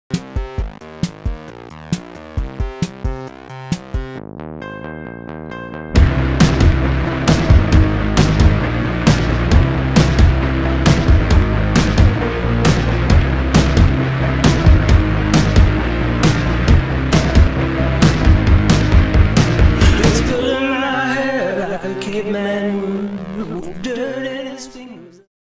Surf Solutions At Wavegarden
Surf Solutions took some of the very best of British junior surfing talent to Surf Snowdonia to try it out as a training facility. Here's a teaser of the full edit which can be found on Magicseaweed.com